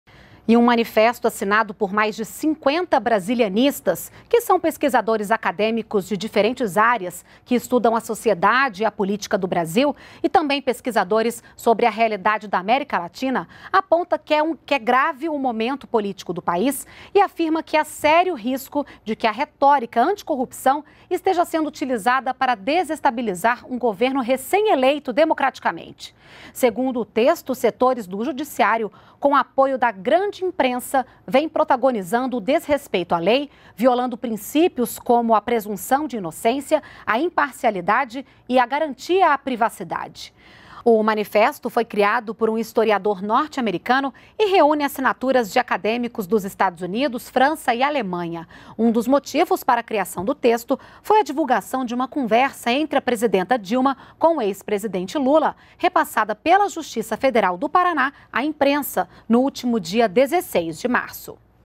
Manifesto assinado por pesquisadores acadêmicos condena desrespeito à democracia
NBR NOTÍCIAS - 29.03.16: Mais de 50 brasilianistas - pesquisadores acadêmicos estrangeiros de diversas áreas - assinaram manifesto, em que consideraram grave o momento político vivido pelo país. De acordo com texto, Judiciário e imprensa têm desrespeitado a lei e princípios constitucionais.